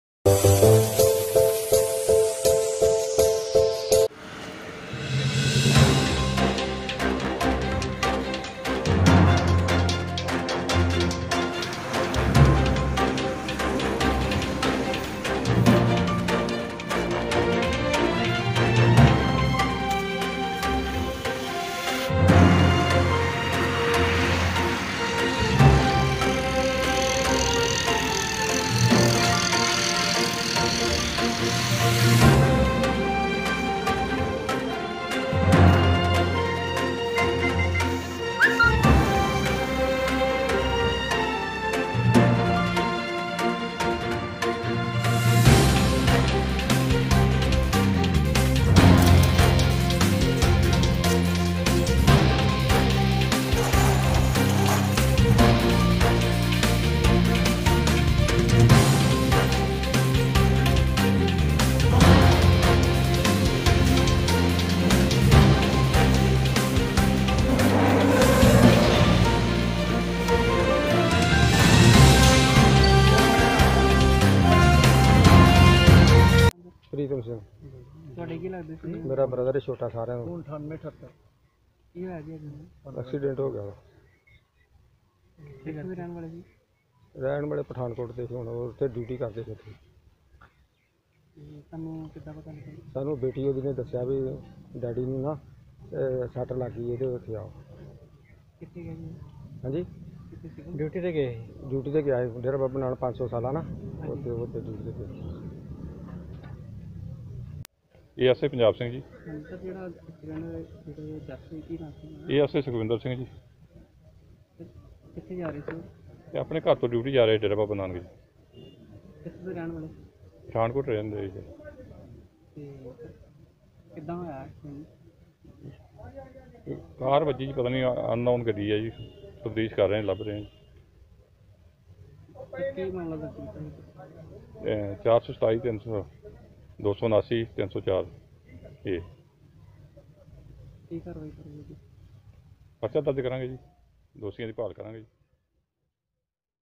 ਸੜਕ ਹਾਦਸੇ ਵਿੱਚ ਰੇਲਵੇ ਪੁਲਿਸ ਮੁਲਾਜਿਮ ਸੁਖਵਿੰਦਰ ਸਿੰਘ ਦੀ ਹੋਈ ਮੌਤ ਡੇਰਾ ਬਾਬਾ ਨਾਨਕ ਵਿਚ ਲੱਗੀ ਸੀ ਡਿਉਟੀ ਪੁਲਿਸ ਨ
ਗੁਰਦਾਸਪੁਰ ਦੇ ਧਾਰੀਵਾਲ ਬਾਈਪਾਸ ਤੇ ਰੇਲਵੇ ਦੇ ਪੁਲਿਸ ਮੁਲਾਜਿਮ ਦੀ ਕਾਰ ਨੂੰ ਅਣਪਛਾਤੇ ਵਾਹਨ ਨੇ ਮਾਰੀ ਟੱਕਰ ਸੜਕ ਹਾਦਸੇ ਵਿੱਚ ਰੇਲਵੇ ਪੁਲਿਸ ਮੁਲਾਜਿਮ ਸੁਖਵਿੰਦਰ ਸਿੰਘ ਦੀ ਹੋਈ ਮੌਤ ਡੇਰਾ ਬਾਬਾ ਨਾਨਕ ਵਿਚ ਲੱਗੀ ਸੀ ਡਿਉਟੀ ਪੁਲਿਸ ਨੇ ਮ੍ਰਿਤਕ ਦੇਹ ਨੂੰ ਕਬਜ਼ੇ ਵਿੱਚ ਲੈਕੇ ਜਾਂਚ ਕੀਤੀ ਸ਼ੁਰੂ k live news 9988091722